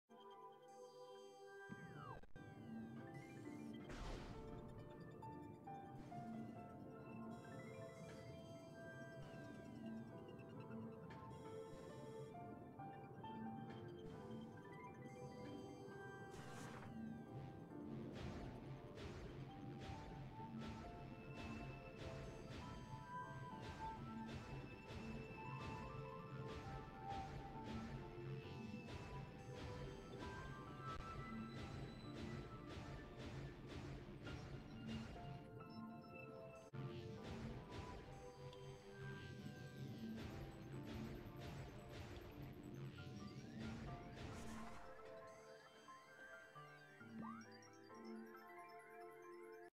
Cargo Freight Lift Gravityless Bombless Boost (NSJ)
Allows the player to climb the room with boost ball.
The first jump requires a fairly precise slope jump off the mossy bit. Once on top, the sloped surface is sticky and you can simply boost up it. Small controlled boosts seems to work best. Reaching the final submerged platform and then boosting out is the trickiest part.